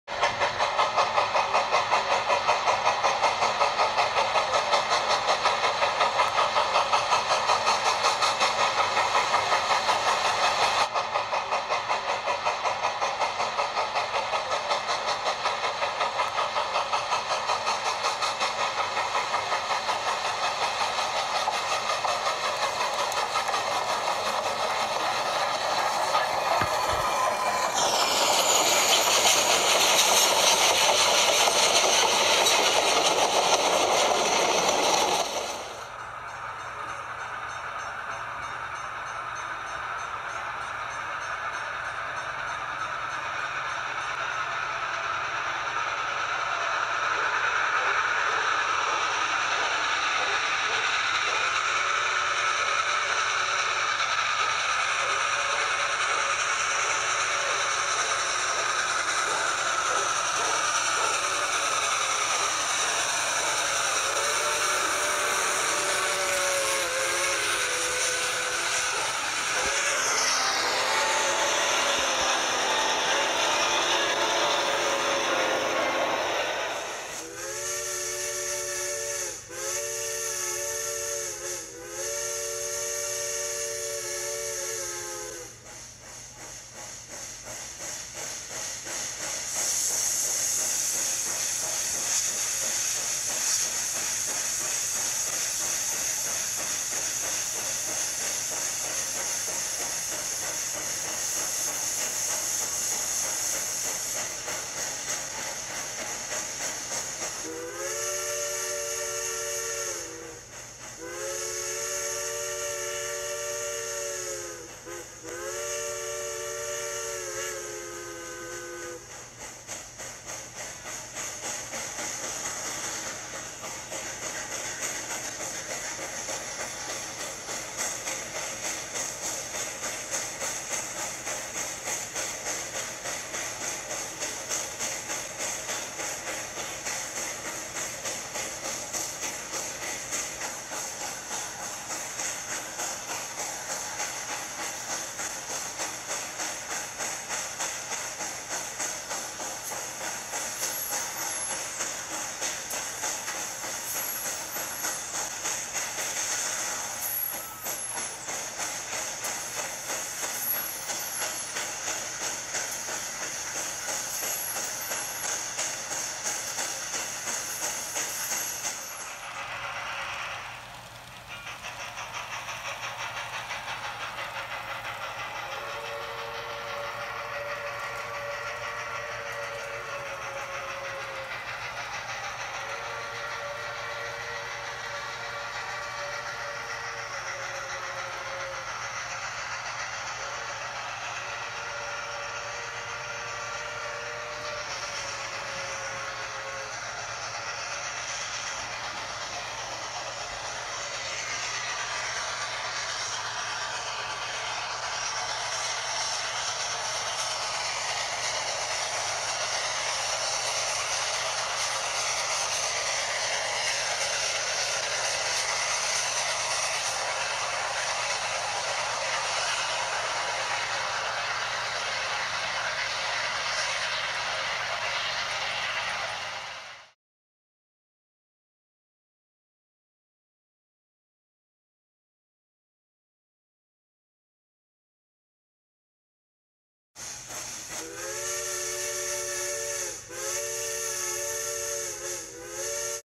Union Pacific 838 - Labor Day Cargo Ride
Union Pacific 838 Pulls 8 Gondola Cars On The Day Before Labor Day